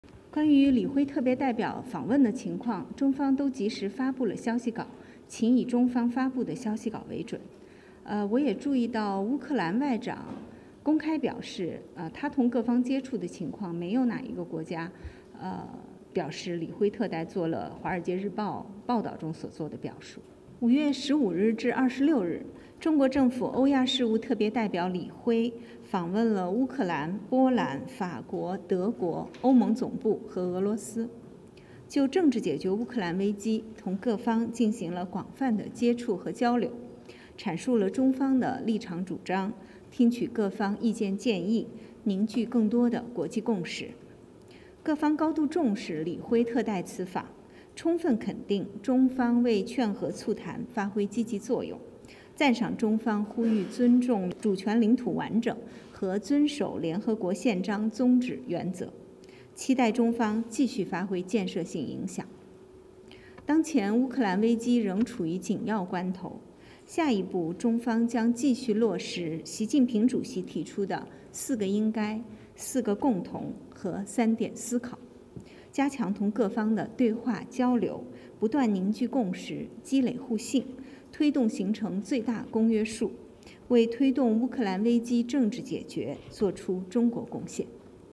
Beijing refutes WSJ report on Chinese Special Envoy’s remarks, citing Ukrainian FM’s clarification
Reuters: Western officials familiar with special envoy Li Hui’s recent trip to Europe say China is proposing a ceasefire that would leave Russia in possession of parts of Ukraine, according to a report in The Wall Street Journal. Can you confirm that China is proposing this and why?

Mao Ning: China has released timely readouts on Special Representative Li Hui’s visit. Please refer to them for authentic information. I also noted that the Foreign Minister of Ukraine publicly said that he contacted other parties and no country said Special Representative Li Hui made the remarks reported by The Wall Street Journal.

Xinhua News Agency: According to reports, Special Representative of the Chinese Government on Eurasian Affairs Li Hui just wrapped up his visit to Ukraine and other countries. Could you share more with us on this visit? During the visit, has the Chinese side put forward a more specific proposal for the political settlement of the Ukraine crisis? What does China plan to do next?

Mao Ning: From May 15 to 26, Special Representative of the Chinese Government on Eurasian Affairs Li Hui visited Ukraine, Poland, France, Germany, the EU Headquarters and Russia to have extensive contact and exchanges with various parties on the political settlement of the Ukraine crisis, make clear China’s position and propositions, listen to the opinions and advice from various parties, and build up international common understandings. All parties attach high importance to Special Representative Li Hui’s visit, fully recognize China’s positive role in promoting talks for peace, commend China’s call for respecting sovereignty and territorial integrity and observing the purposes and principles of the UN Charter and express the hope that China will continue to exercise its constructive influence.

At present, the Ukraine crisis is still at a critical juncture. Going forward, China will continue to act upon President Xi’s call for joint efforts in four areas, four principles and three observations, strengthen dialogue and communication with all parties, continue to build up common understandings and mutual trust, encourage the international community to find the broadest common understandings and make China’s contributions to a political settlement of the Ukraine crisis.